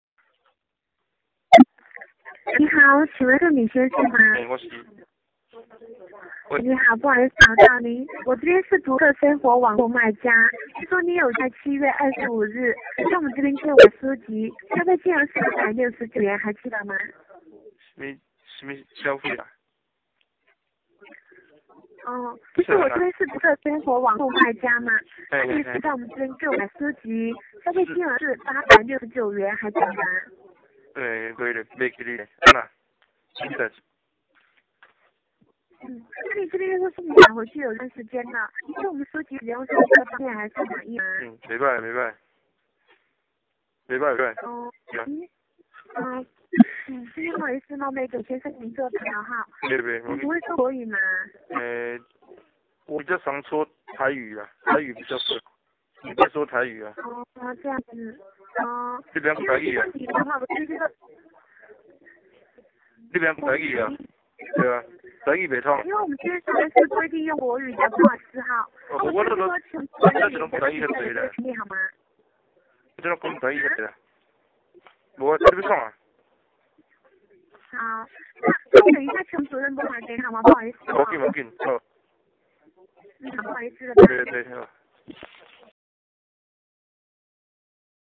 Taaze 讀冊生活 詐騙電話
20110827
 Taaze  讀冊生活詐騙電話
台語與大陸人的對話